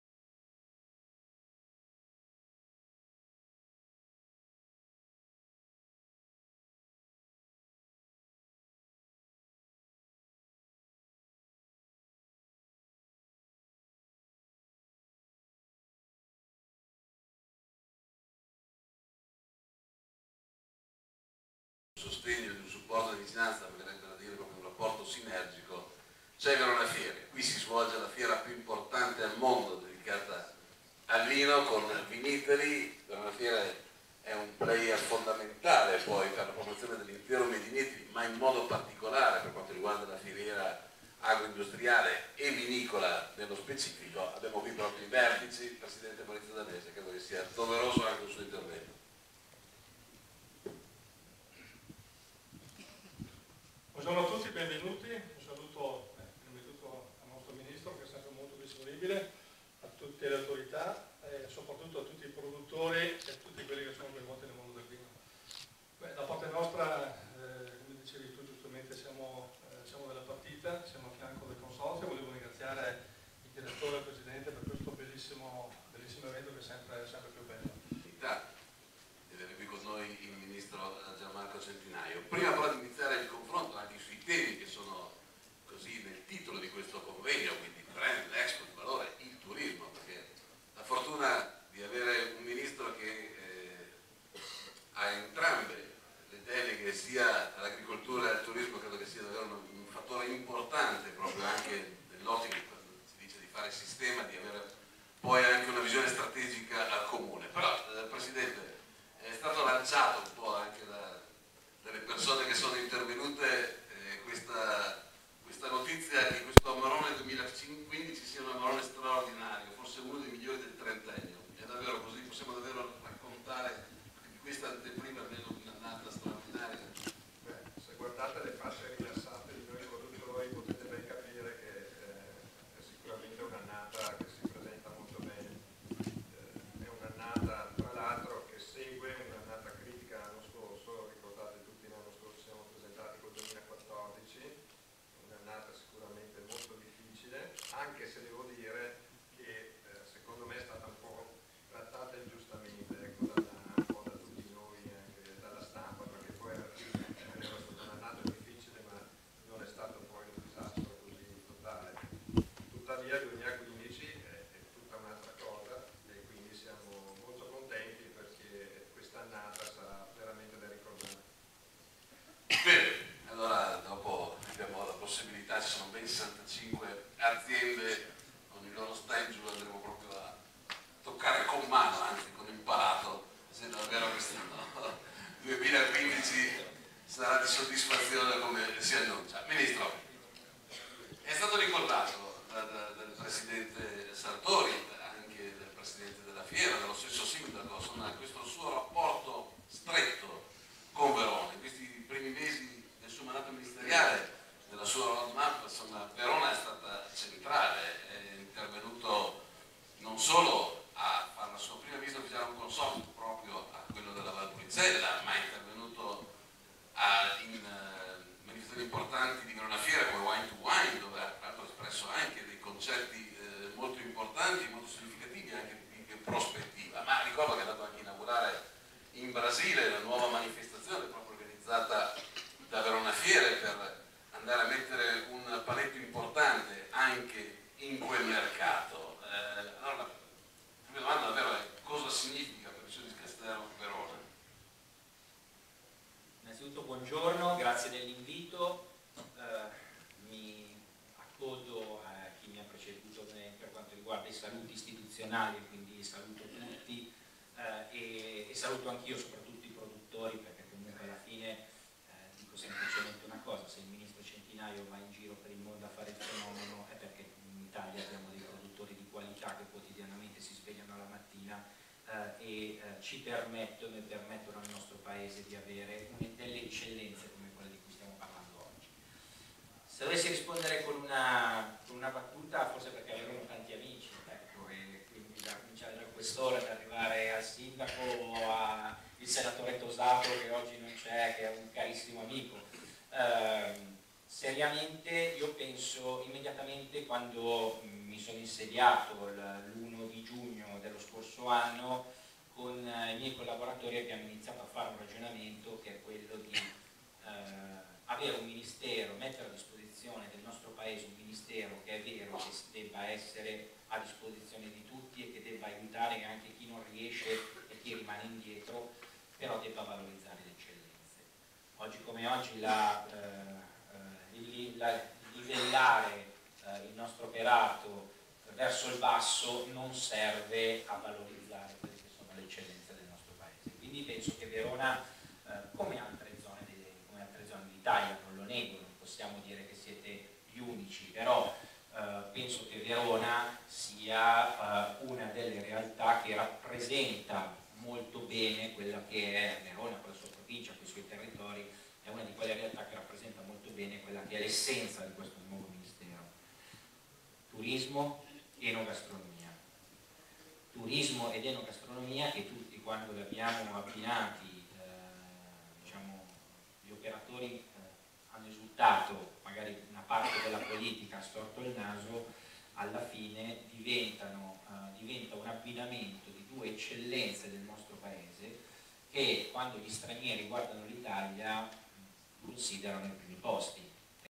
Conferenza di apertura Anteprima Amarone 2015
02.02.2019 Palazzo della Guardia a Verona - Anteprima Amarone 2015 - Conferenza di apertura di Anteprima Amarone 2015 a Verona.
Moderatore Andrea Andreoli, giornalista - Maurizio Danese, presidente Vinitaly - Andrea Sartori, residente del Consorzio Tutela Vini della Valpolicella - Sen. Gian Marco Centinaio, Ministro Agricoltura e turismo. Parte tecnica, Giovanni Tavassi.